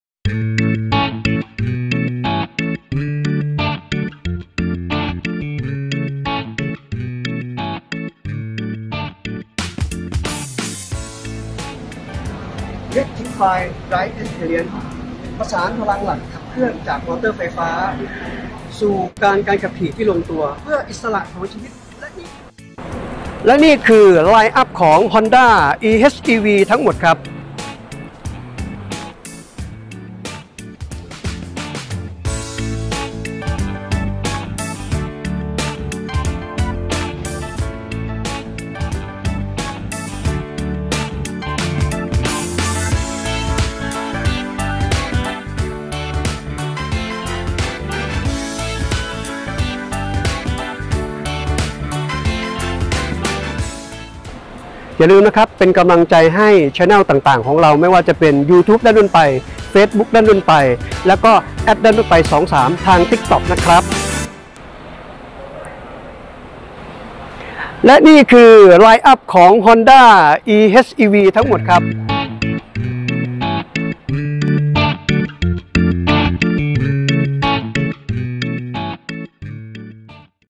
Honda e:HEV System Full-Hybrid ประหยัดน้ำมันดีเยี่ยม ขับได้ไกล เร่งแรงแซงตามคิด
⚡พบกับขุมพลัง e:HEV ในรถยนต์ทุกรุ่นของ Honda ที่งาน Motor Expo 2023
⚡ระหว่าง 29 พฤศจิกายน ถึง  11 ธันวาคม 2566  ชาเลนเจอร์ เมืองทองธานี
⚡หรือที่โชว์รูมฮอนด้าทั่วประเทศไทย ตั้งแต่วันนี้เป็นต้นไป